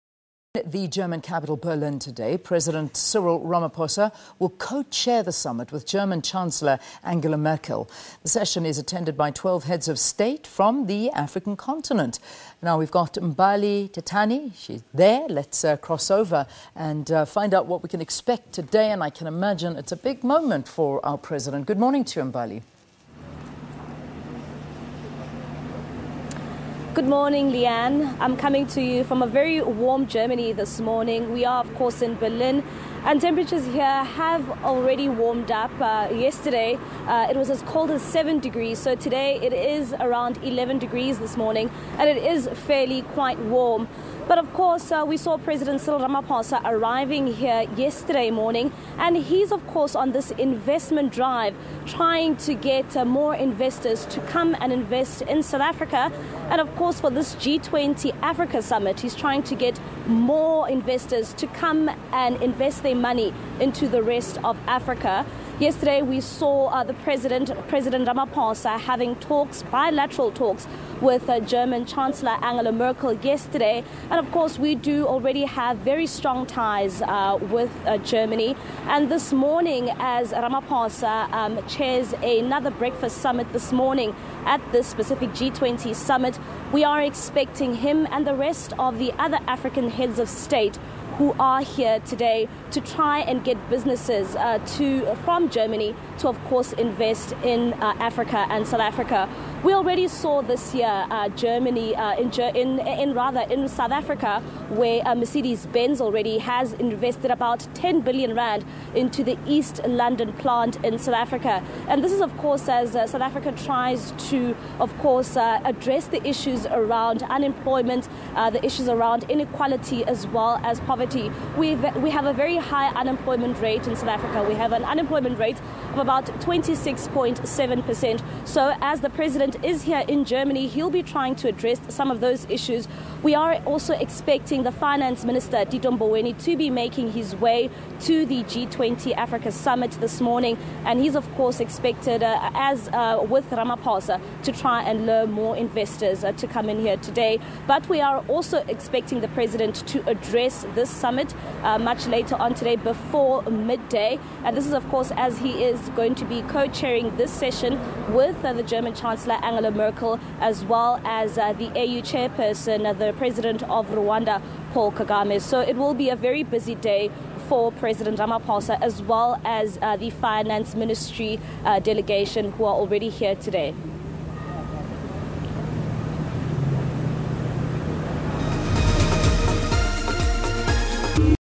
G20 Africa Investment Summit
The G20 Africa Investment Summit gets underway in the German capital,  Berlin, today.
President Cyril Ramaphosa will co-chair the Summit with German Chancellor Angela Merkel.
This session is attended by 12 heads of state from the Africa continent.
Let's cross to our Mbali Thethani.